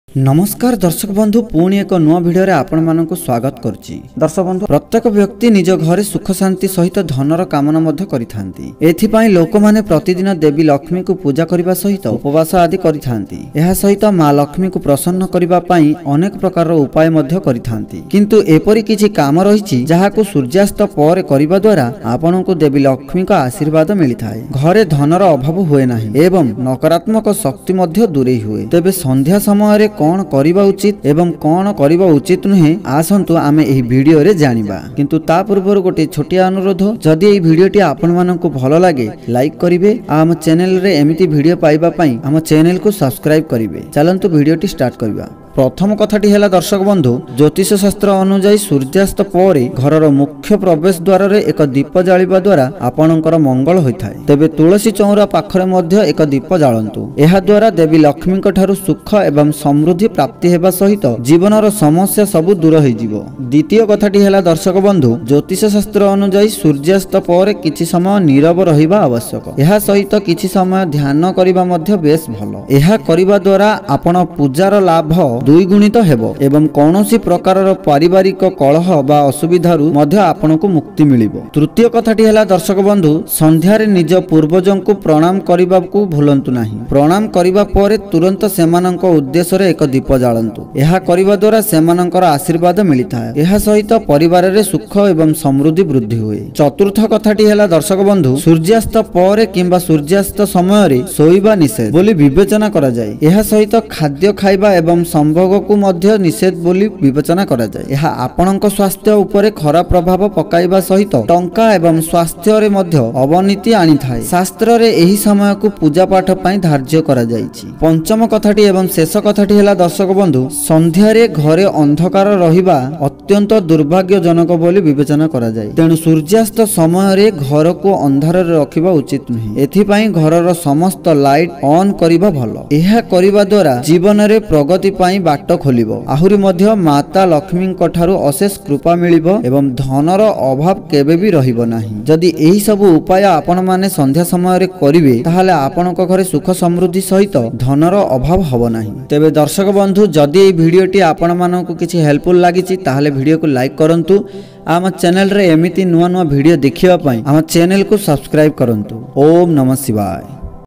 Odia Vastu tips | Vastu tips in odia | ajira Anuchinta | Sadhu Bani | vastu tips for money
About this video:
Ajira ehi video re janiba Sandhya samaya re karantu ehi kama haba dhanara barsha. Vastu tips odia re